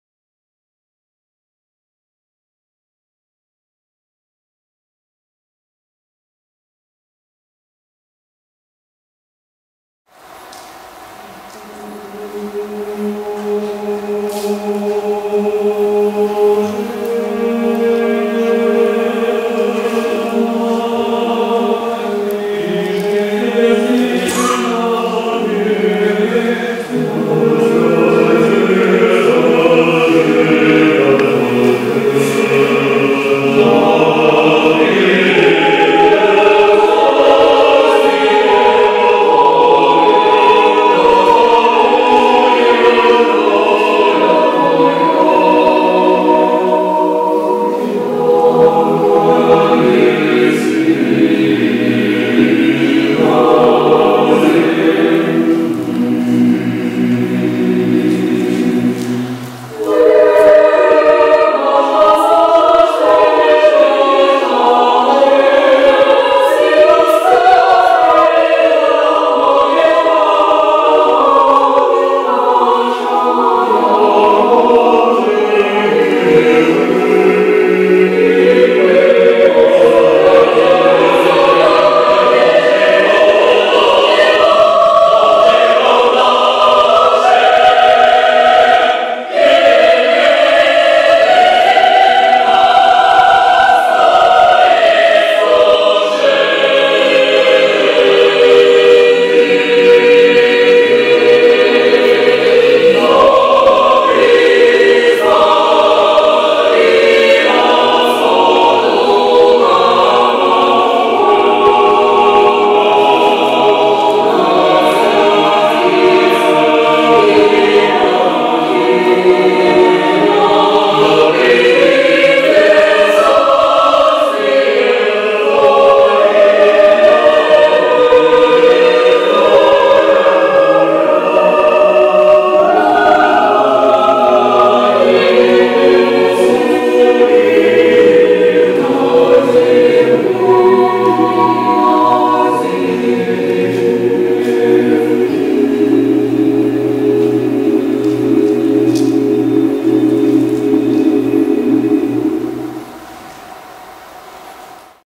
Chant du très Saint Théotokos 2019 11 15
Le chœur de Crimée à l’invitation de la chorale Mélimélodie de la Forest-Landerneau, de l’amicale Laïque de la Forest Landerneau et de l’association Peuples solidaires de Landerneau.